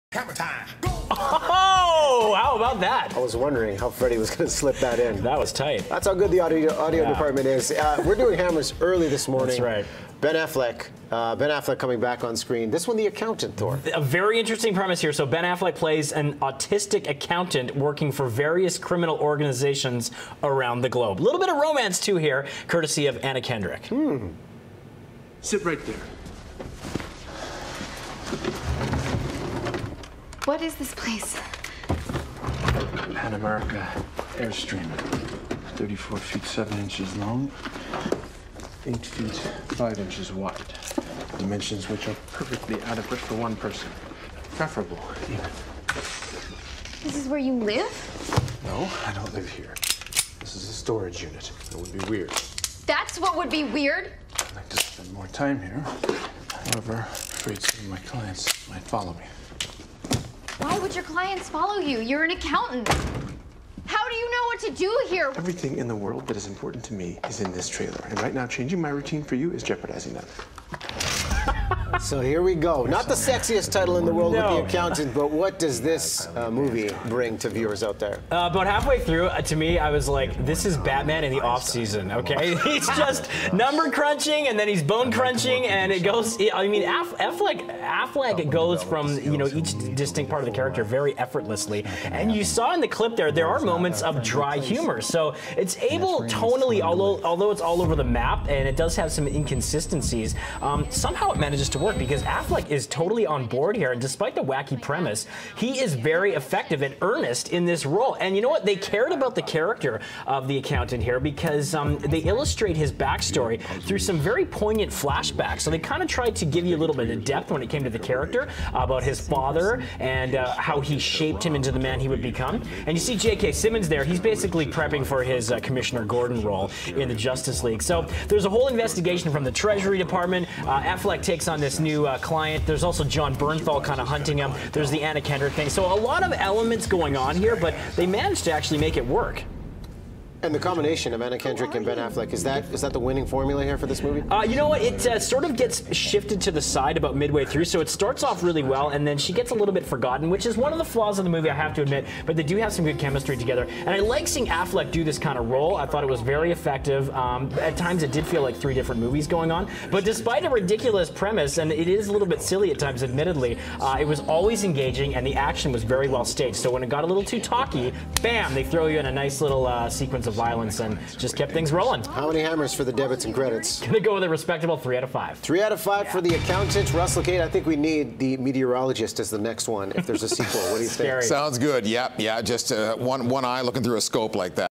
Thor's Hammers: The Accountant
Ben Affleck ditches the cape for a desk job. Thor drops the Hammers on the new thriller ‘The Accountant’.